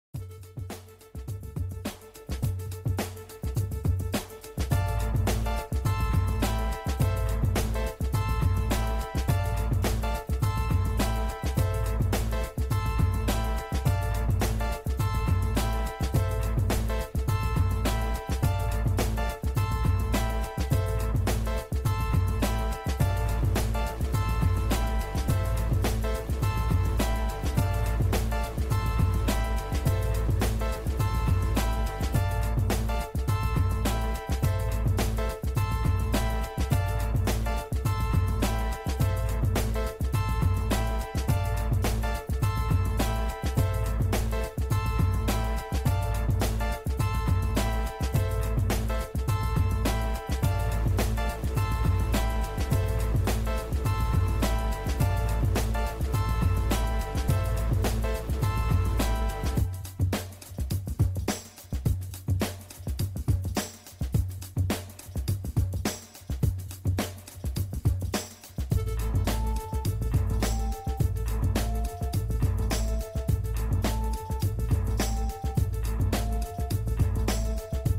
2010 Dodge Challenger Black Virginia Beach VA
Autos & Vehicles

Year: 2010
Make: Dodge
Model: Challenger
Ext Color: Black
Int Color: Dark Slate Gray

Dealer's Name: Charles Barker Toyota
Dealer's City: Virginia Beach
Dealer's State: VA
Dealer's zip: 23454

Cities where people might be interested in this vehicle as well:
Virginia Beach

2B3CJ4DV9AH306748